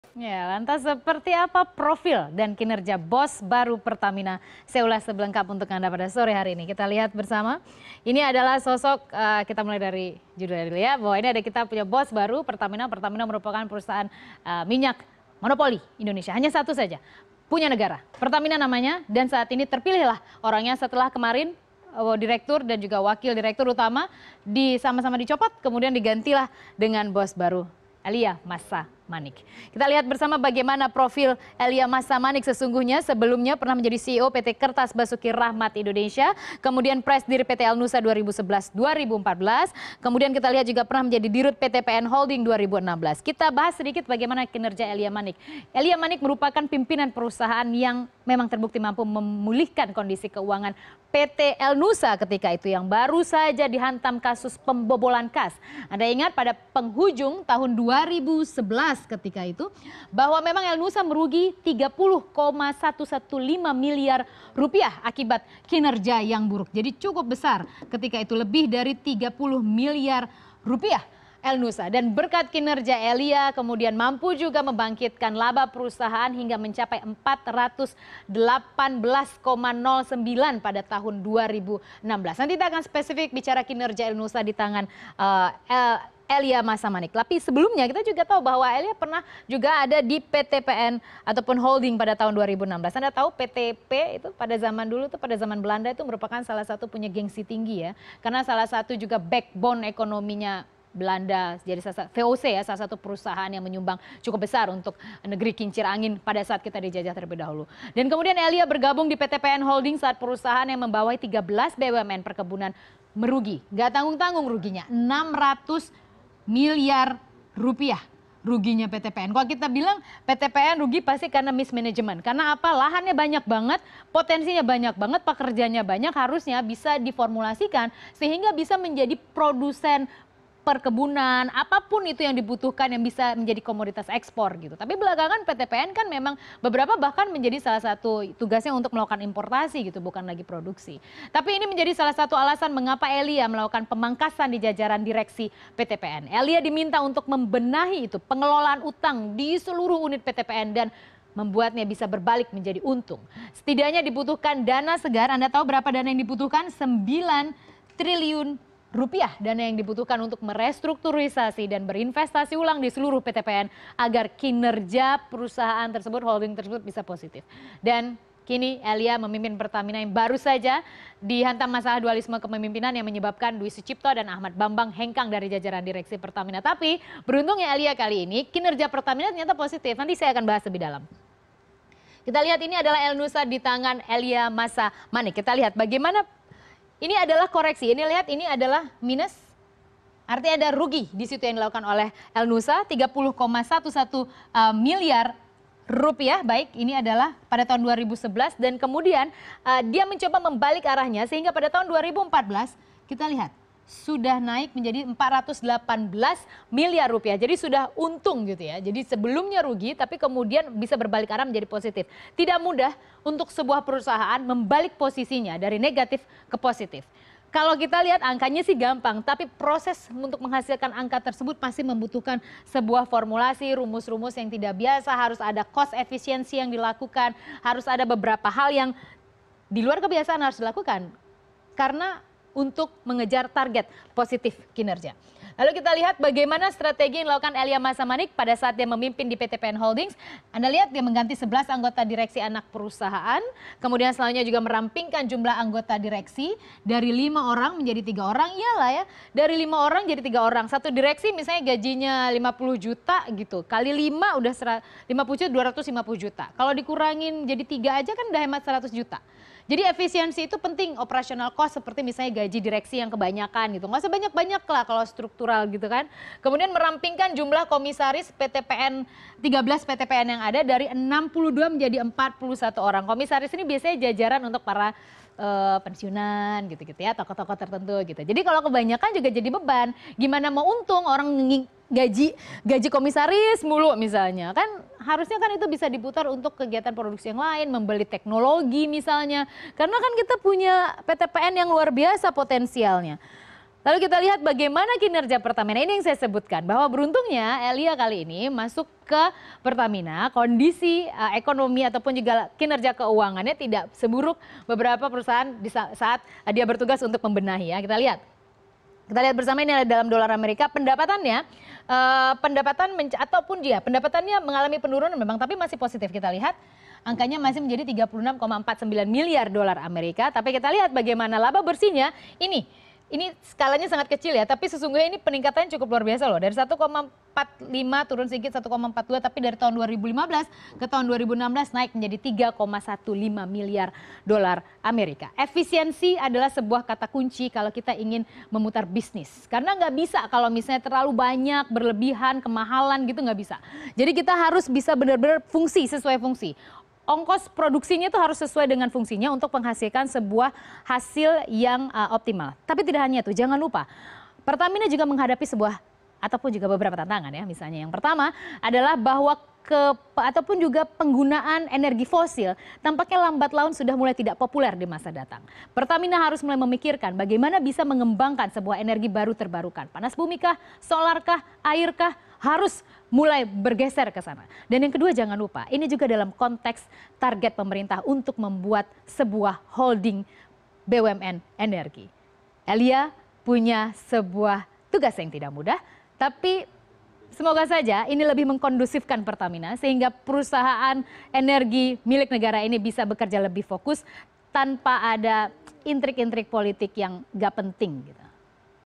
Ini Profil & Kinerja Bos Baru Pertamina
Elia Massa Manik resmi menjadi Direktur Utama baru PT Pertamina. Sepertia apa profil dan kinerja bos baru Pertamina tersebut. Berikut akan kami ulas untuk anda bersama Hera f. Haryn.